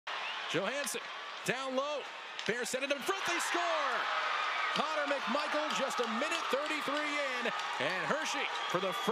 GOAL Connor McMichael Firebirds v Hershey Bears 5-2 AHL 2022-23#shorts
GOAL. Connor McMichael. Coachella Valley Firebirds v Hershey Bears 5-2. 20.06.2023